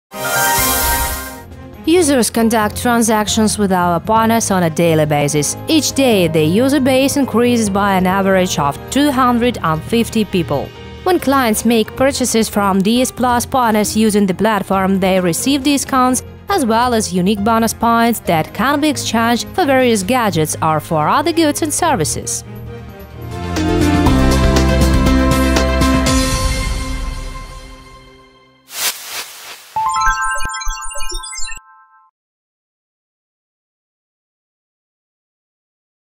Quote from PlusCoin Whitepaper
Users conduct transactions with our partners on a daily basis. Each day, the user base increases by an average of 250 people. When clients make purchases from DS PLUS partners using the platform, they receive discounts, as well as unique bonus points that can be exchanged for various gadgets, or for other goods and services.